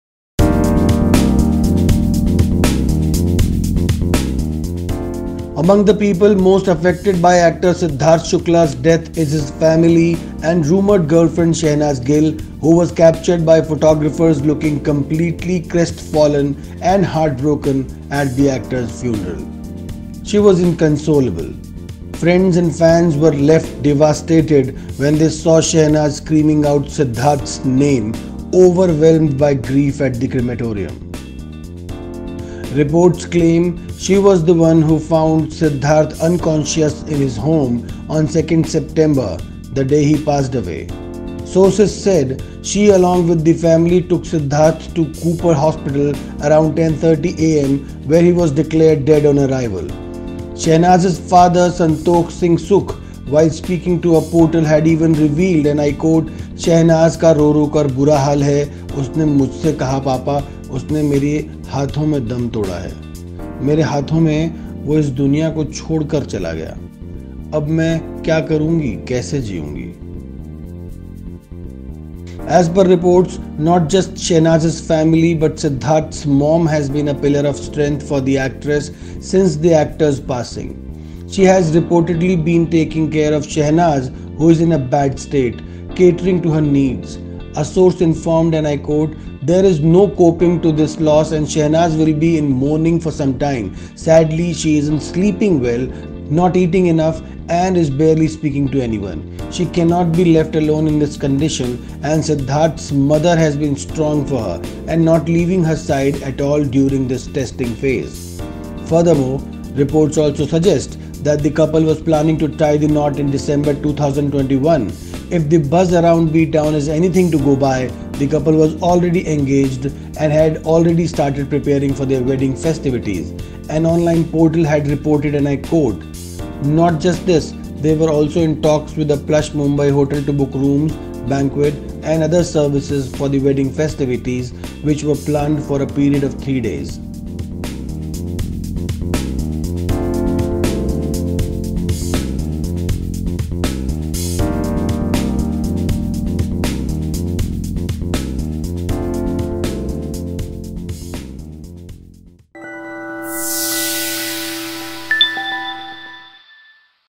Sidharth Shukla's death: Shehnaaz Gill is still in a state of shock
Among the people most affected by actor Sidharth Shukla's death is his family and rumoured girlfriend Shehnaaz Gill, who was captured by photographers looking completely crestfallen and heartbroken at the actor's funeral. She was inconsolable. Friends and fans were left devastated when they saw Shehnaaz screaming out Sidharth's name, overwhelmed by grief at the crematorium. Reports claim she was the one who found Sidharth unconscious in his home on 2 September, the day he passed away. Sources said she, along with the family, took Sidharth to Cooper hospital around 10:30 am where he was declared dead on arrival. Shehnaaz's father, Santokh Singh Sukh, while speaking to a portal, had even revealed, 'Shehnaaz ka ro rokar bura haal hai. Usne mujhse kaha “papa, usne mere haathon mein dum toda hai. Mere haathon mein woh is duniya ko chhodkar gaya. Ab main kya karungi kaise jeeyungi?' As per reports, not just Shehnaaz's family, but Sidharth's mom has been a pillar of strength for the actress since the actor's passing. She has reportedly been taking care of Shehnaaz who is in a bad state, catering to her needs. A source informed, 'There is no coping to this loss and Shehnaaz will be in mourning for sometime. Sadly, she isn't sleeping well, not eating enough and is barely speaking to anyone. She cannot be left alone in this condition and Sidharth's mother has been strong for her, and not leaving her side at all during this testing phase.' Furthermore, reports also suggest that the couple was planning to tie the knot in December 2021. If the buzz around B-Town is anything to go by, the couple was already engaged and had already started preparing for their wedding festivities. An online portal had reported and I quote, 'Not just this, they were also in talks with a plush Mumbai hotel to book rooms, banquet, and other services for the wedding festivities which were planned for a period of three days.'